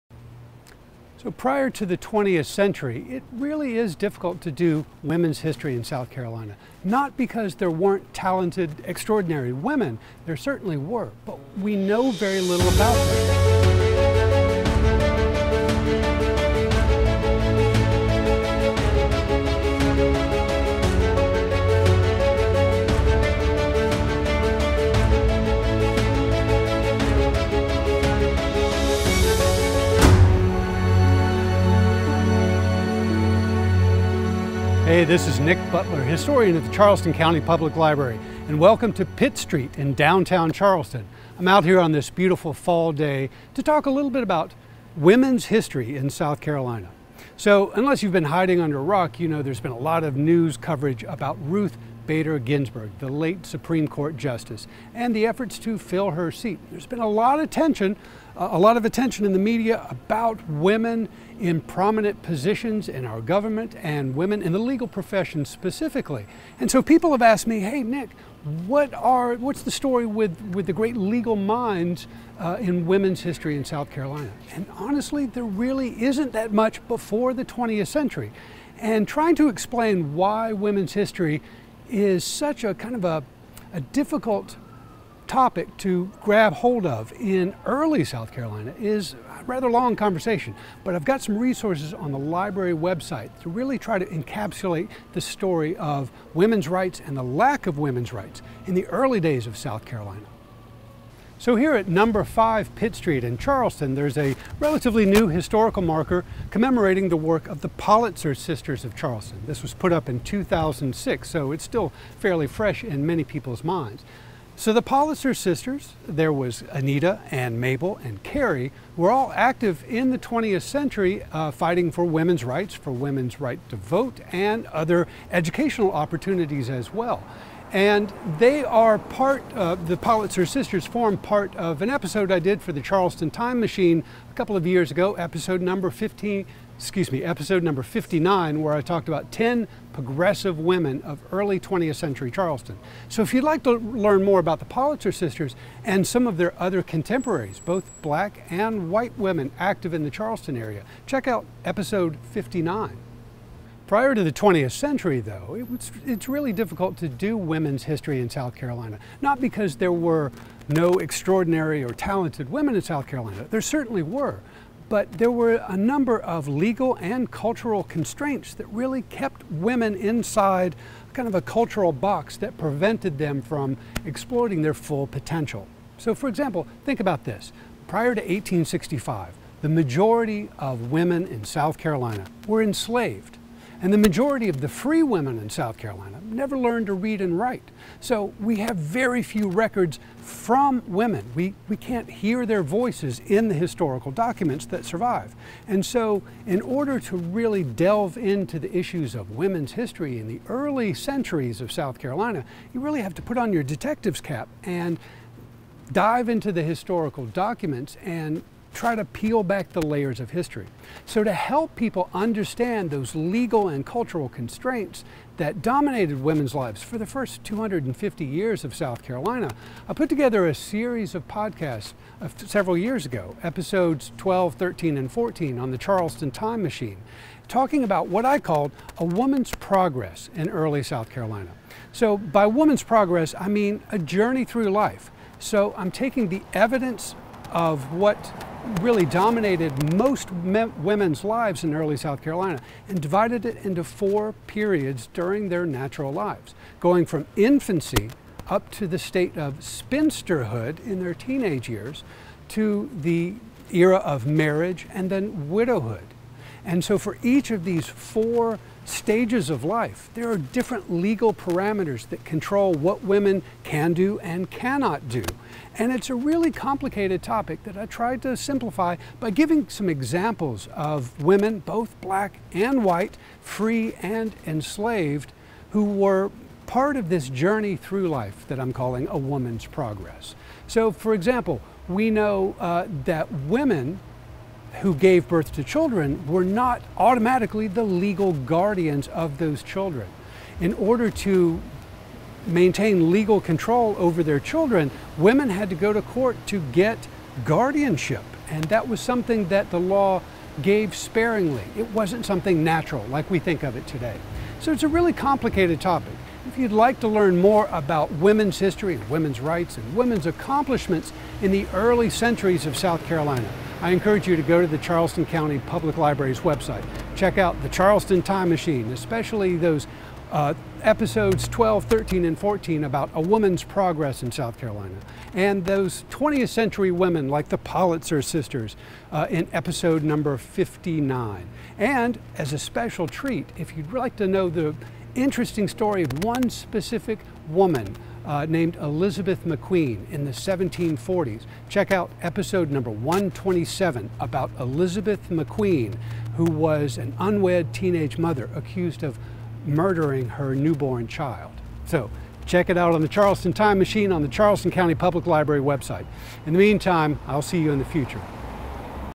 Ask Dr. Nic (s2 Episode 6) - Who were the Pollitzer sisters?
In today's episode of Ask Dr. Nic, we head to Pitt Street in downtown Charleston to talk about women's history, the Pollitzer sisters, and their efforts to give women the right to vote and access to education.

To learn more, check out Episode 59 of the Charleston Time Machine

And check out a woman's progress in Charleston's history starting in episode 12